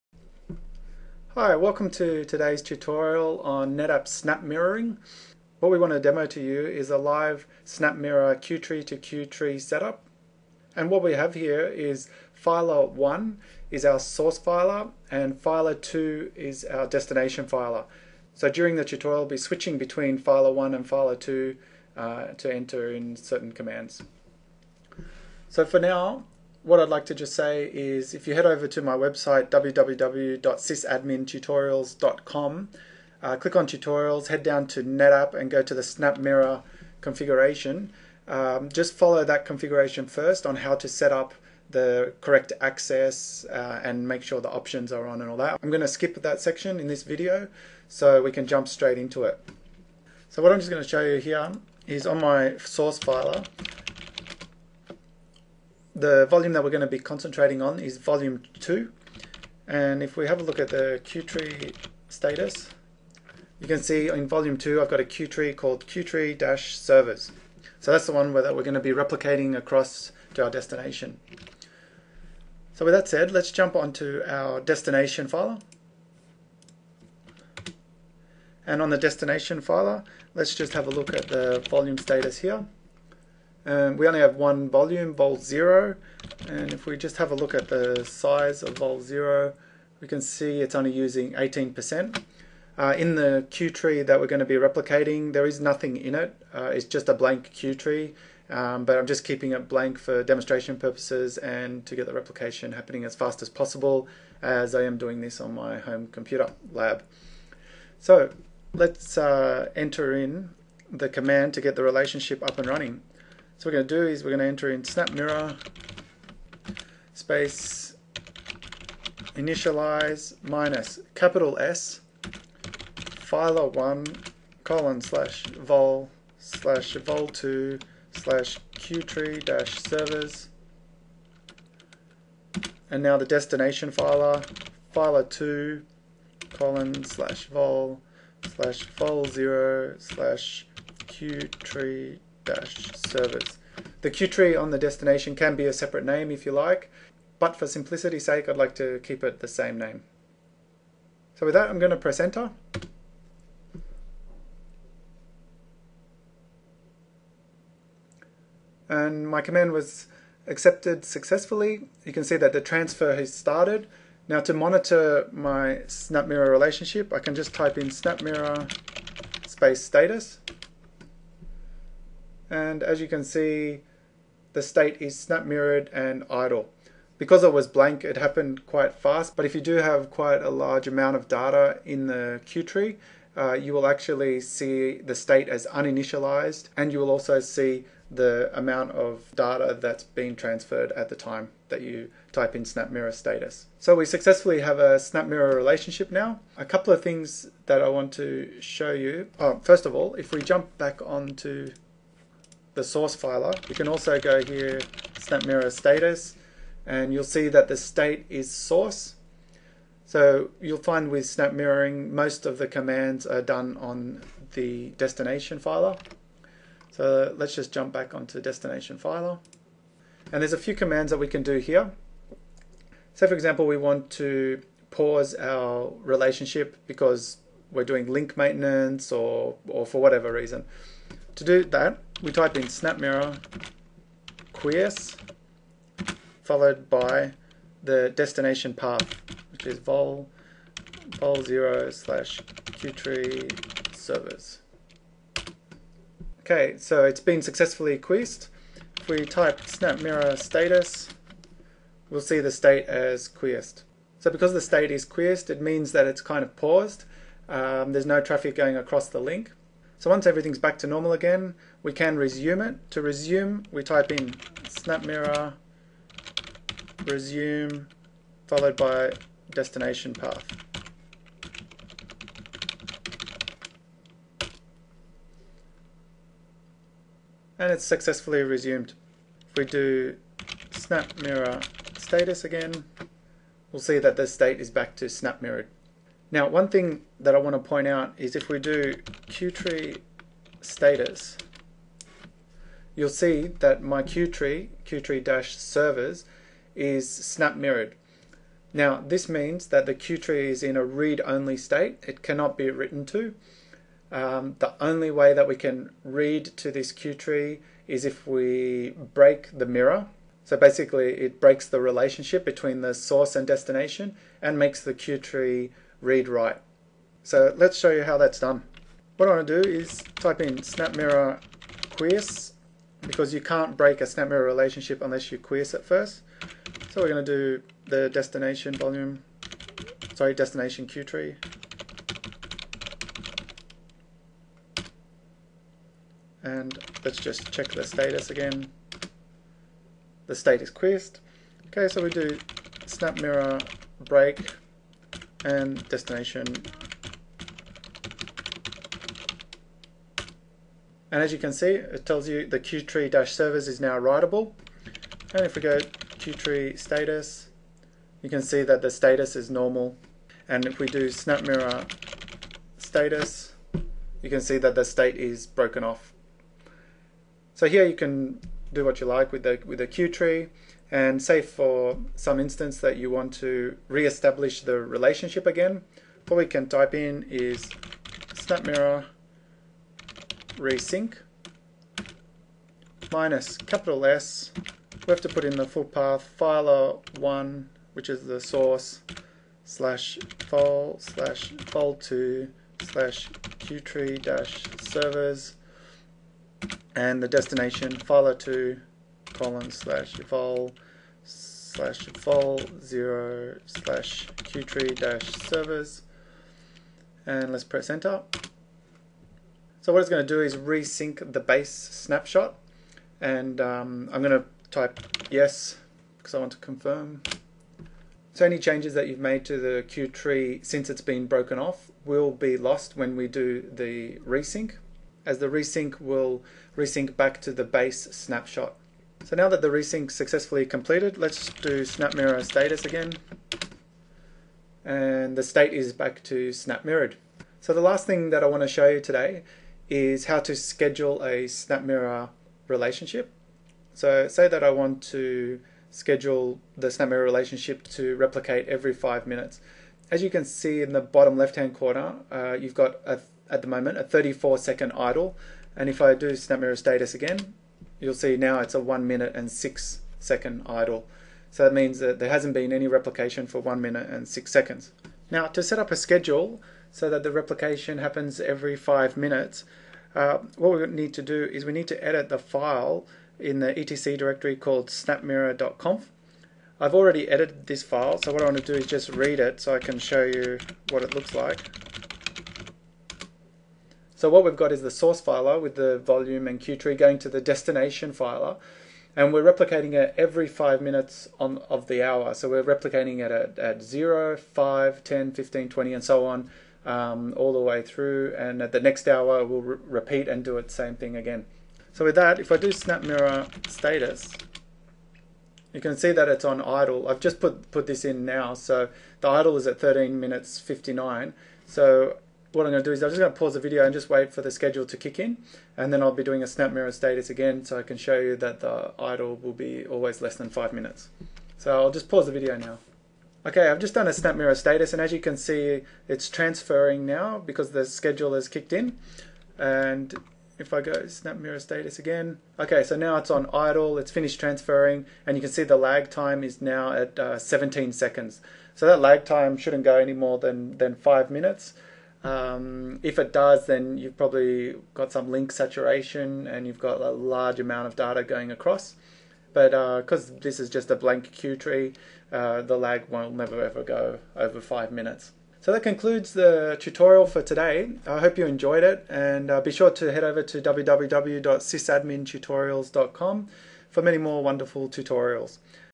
Netapp - Snapmirror Setup, Configuration and Schedule
Learn how to setup, configure and schedule a Snapmirror relationship between a primary and secondary Netapp filer.